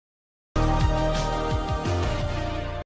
live wallpaper of Mercedes Benz logo
A live background of Mercedes for it's lovers ✌️
 ( download it through Utube bot in telegram and set it for lock screen because when logo appears, video stops and you will have a great view of it)